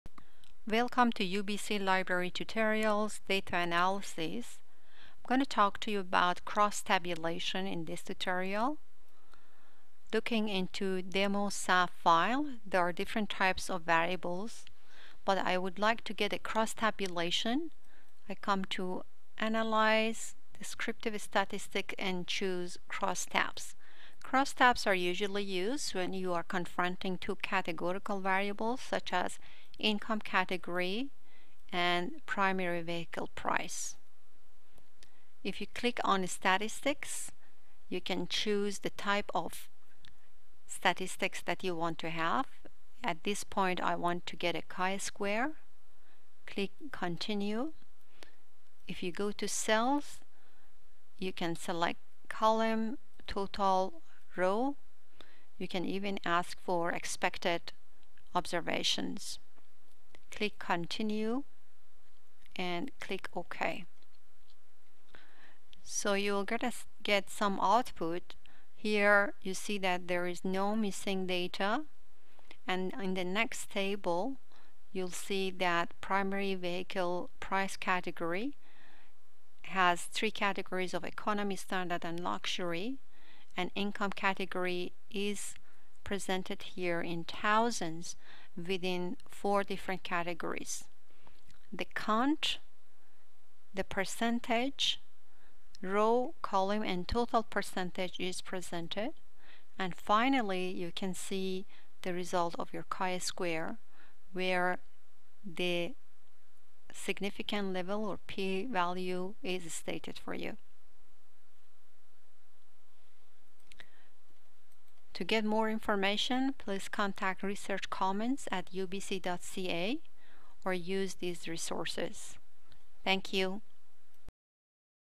SPSS Tutorials: Cross Tabulation
How to create Cross Tabulation of two categorical variables, Chi-square test